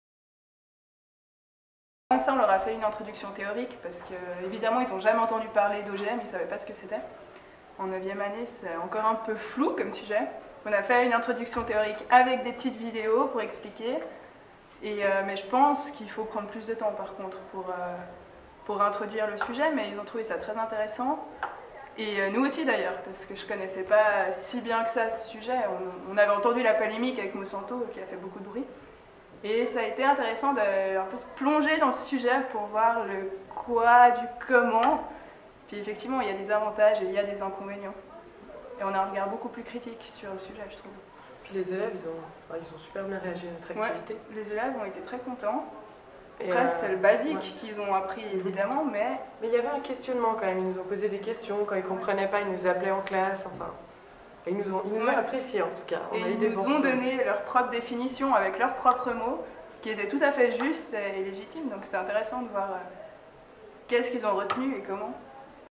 Projet "Agis pour ton futur" - Notre activité sur le thème des OGM
Nous avons élaboré un dossier et des activités pédagogiques sur le thème des OGM et les avons présentés dans une classe de secondaire 1 proche du Collège du Burrier (VD).